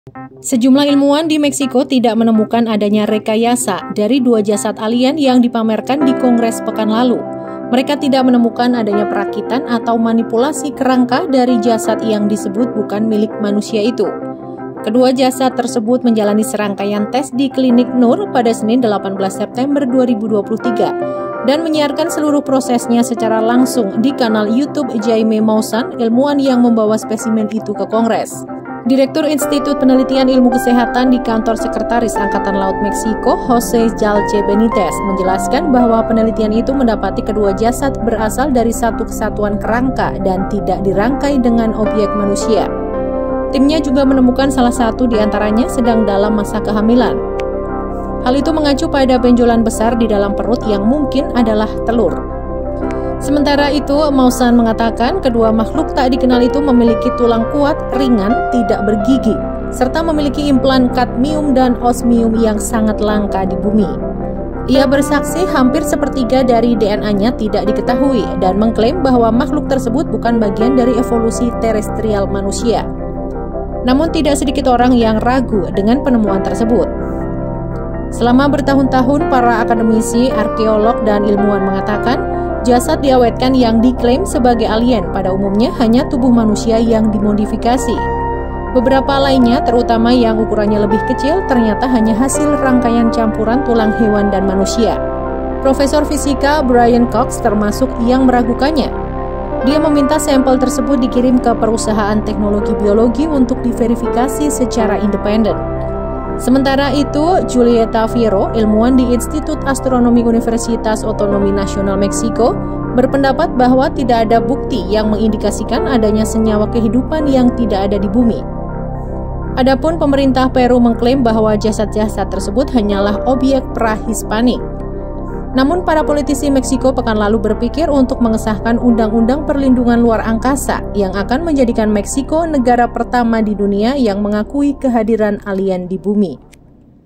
'JASAD ALIEN' DITELITI, ILMUWAN TAK TEMUKAN ADANYA REKAYASA
Sejumlah ilmuwan di Meksiko tidak menemukan adanya rekayasa dari dua "jasad alien" yang dipamerkan di kongres pekan lalu.

Mereka tidak menemukan adanya perakitan atau manipulasi kerangka dari jasad yang disebut bukan milik manusia itu. (*)

Editor : Mona Kriesdinar
Video : Afifudin
Narator : Prisca Ruri
Produser : Ribut Raharjo
(Fif)

Sumber : kompas